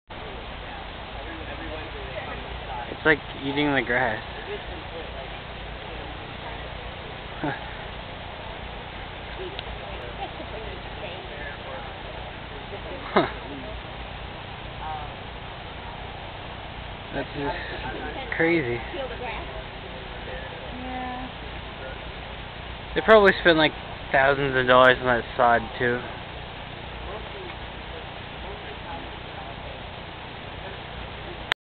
Tiger Eats Grass
A white Tiger at Las Vegas's Secret Garden decides to devour some landscaping